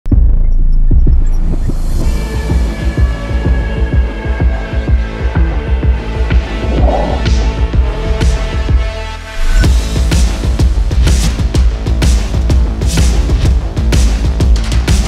Don't Just Go, Go Farther. Trojan Lithium Golf Cart Batteries are here.
Don't just go. Go farther. Trojan's GC2 48V Lithium-Ion Battery is here to change your ride. Release the Beast!